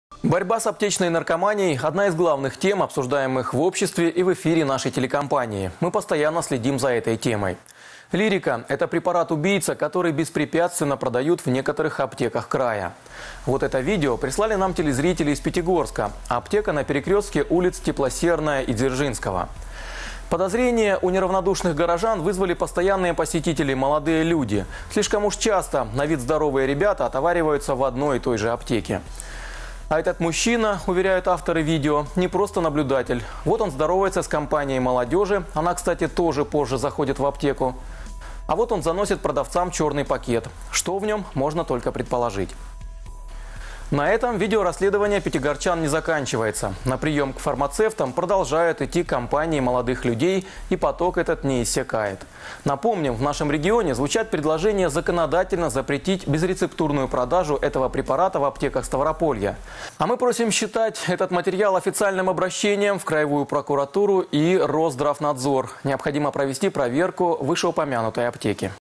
Общество подключилось к борьбе с аптечной наркоманией
Борьба с аптечной наркоманией одна из главных тем, обсуждаемых в обществе и в эфире нашей телекомпании. Мы постоянно следим за этой темой. "Лирика" - препарат-убийца, который беспрепятственно продают в некоторых аптеках края. Вот это видео прислали нам телезрители из Пятигорска. Аптека на перекрёстке улиц Теплосерная и Дзержинского. Подозрение у неравнодушных горожан вызвали постоянные посетители - молодые люди. Слишком часто, на вид - здоровые ребята, "отовариваются" в одной и той же аптеке.